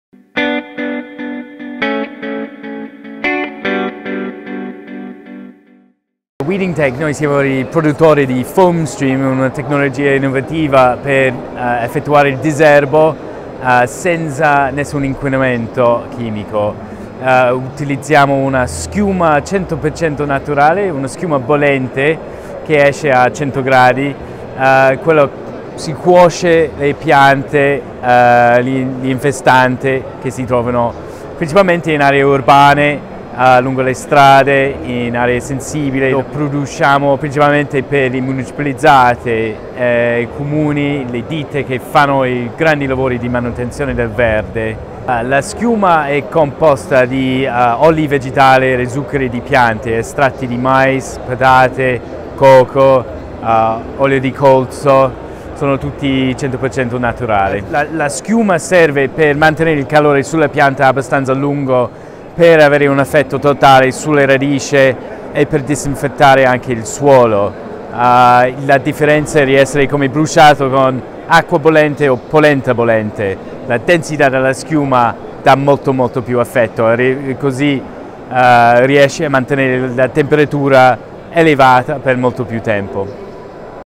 Weddingtech presenta diserbo senza chimica a Ecomondo 2016
Weddingtech presenta Foamstream per effettuare il diserbo senza inquinamento chimico